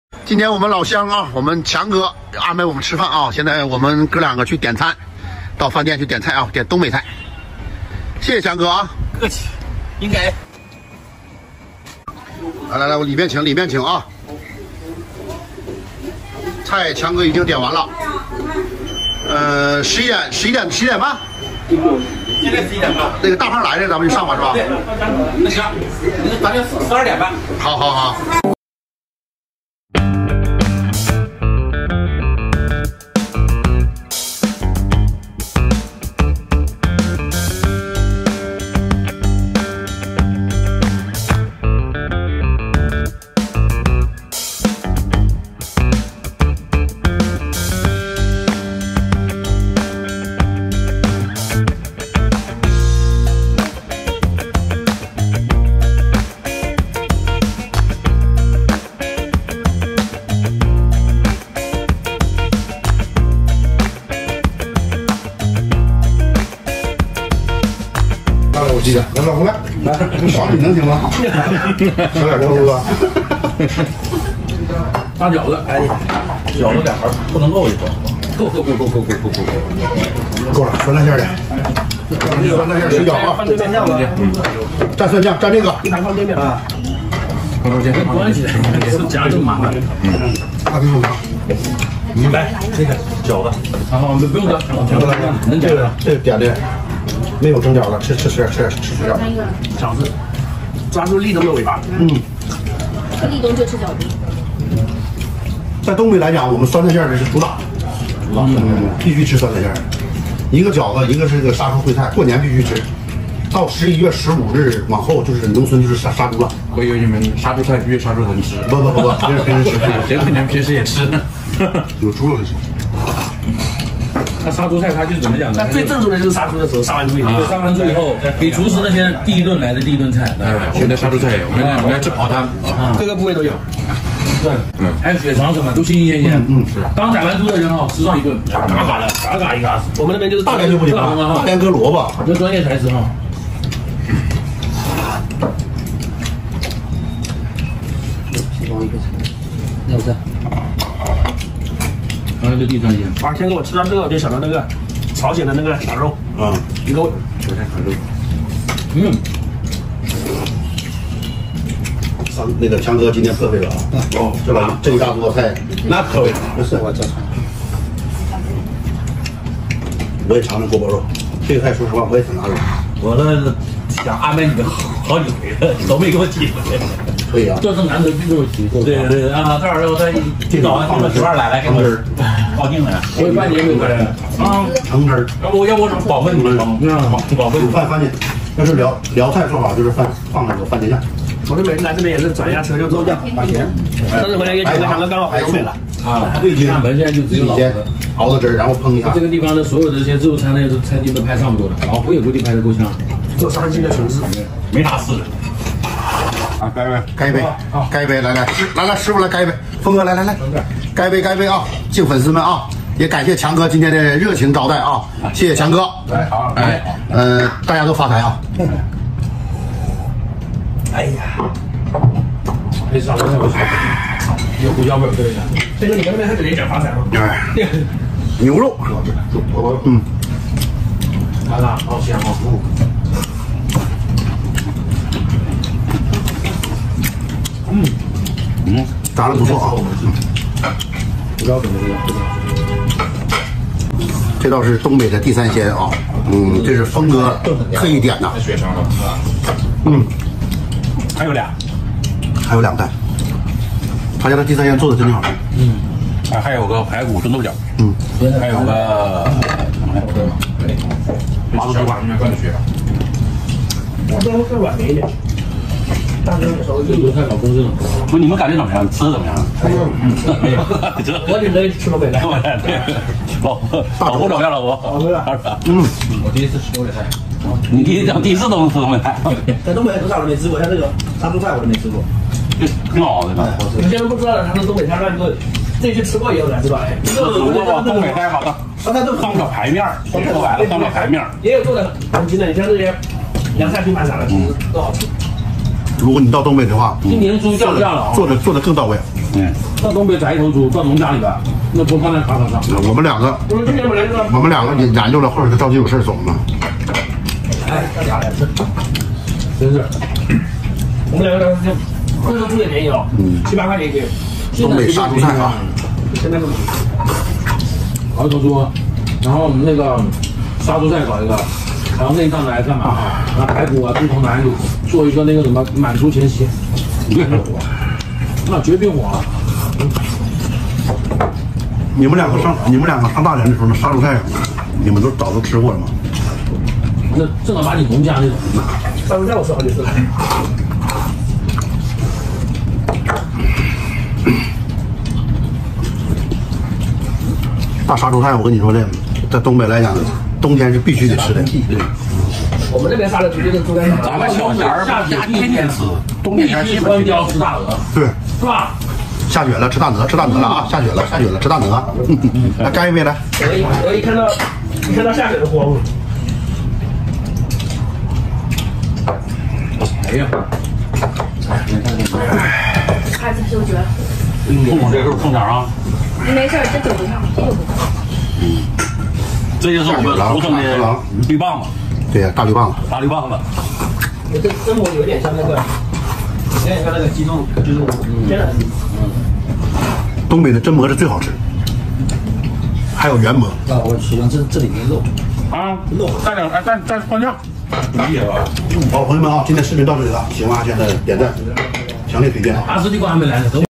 【胖子阿谦】厦门粉丝安排吃饭，十二道菜消费一千多，众兄弟吃的满嘴流油，香
哈喽 大家好！我是胖子阿谦，带你吃尽天下美食！
我不是大厨，能吃会做，走遍天下不挨饿！